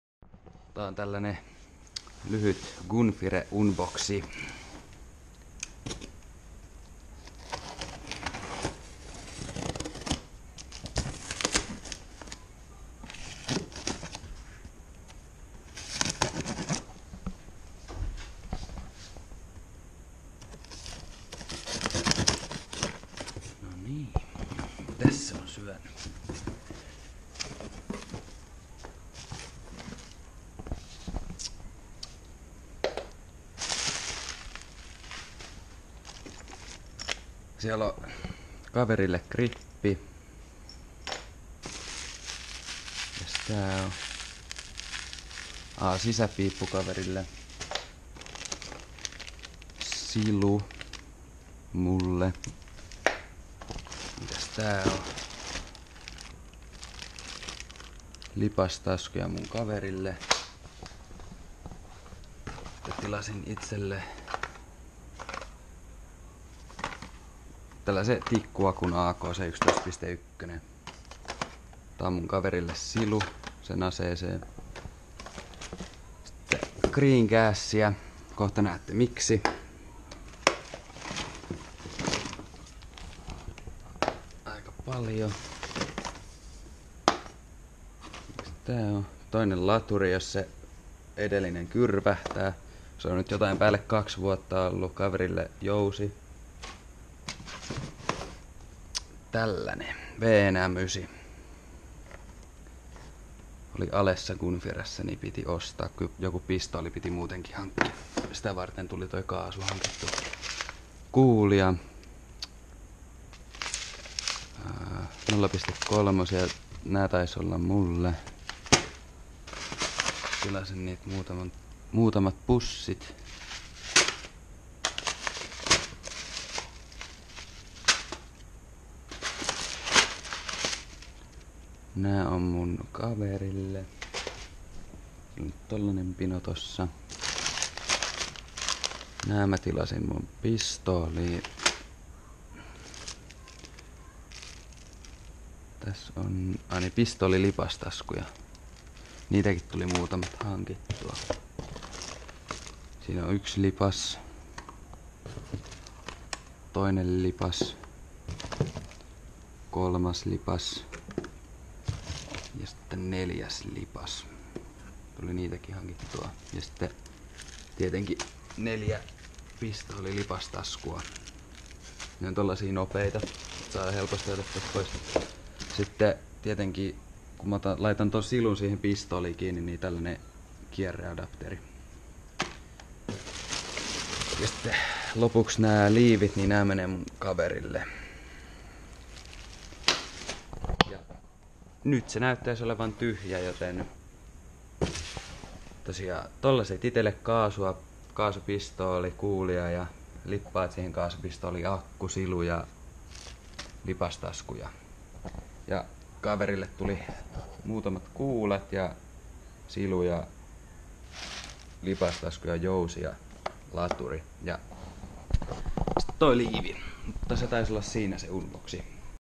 Airsoft Gunfire 400€ Unboxing
Videolla avataan Gunfiresta tullut paketti :)